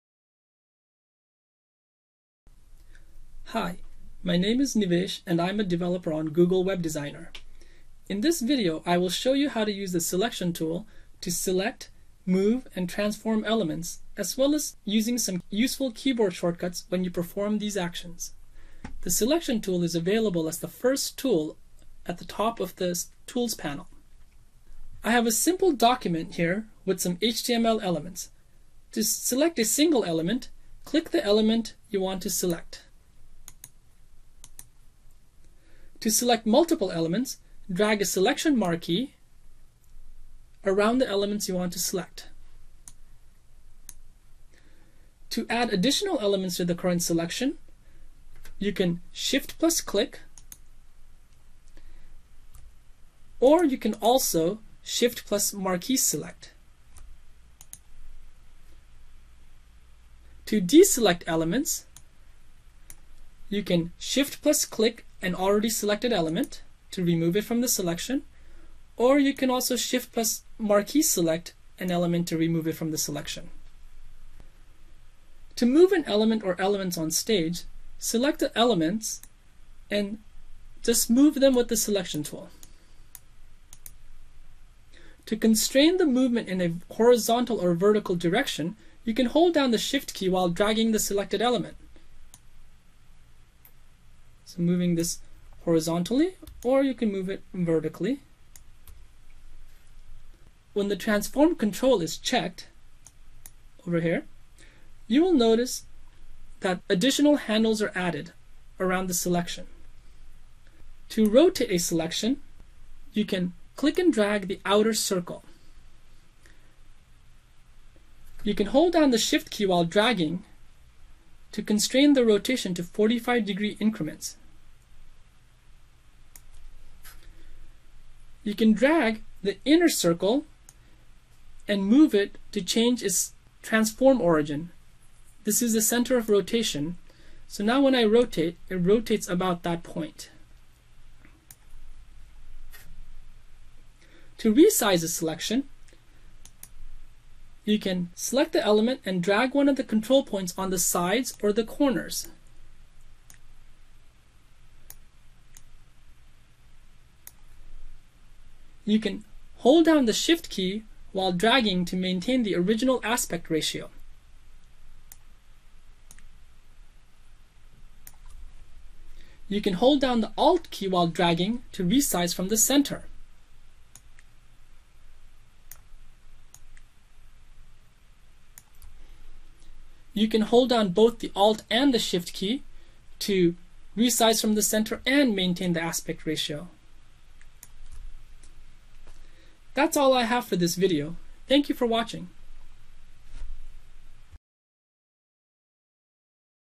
Selection Tool - Google Web Designer
In this video, Google Web Designer engineer Nivesh provides an overview of the Selection tool. Learn how to use the selection tool to select, move, resize, or transform elements.